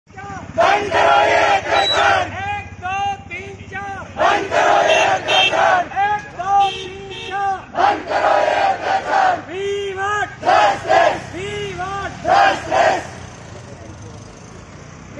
RG Medical College में डॉक्टरों की हालात खराब..🥺😥 Resident Doctors Protest Against the Accused 🥺💔
RG Medical College में डॉक्टरों की हालात खराब..🥺😥 Resident Doctors Protest Against the Accused 🥺💔doctor